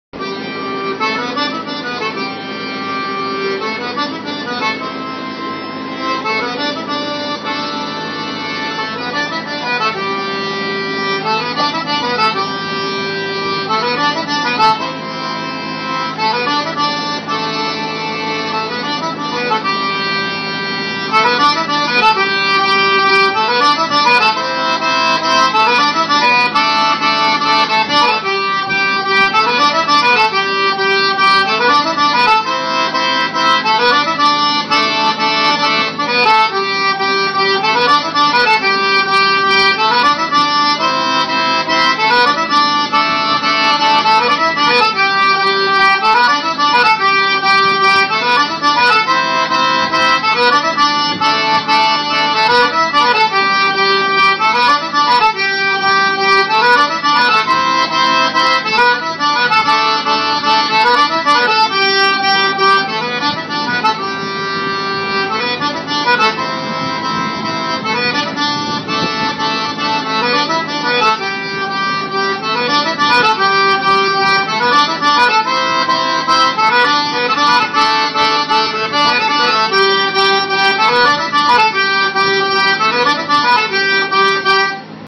Me
Just messin around on da squeezebox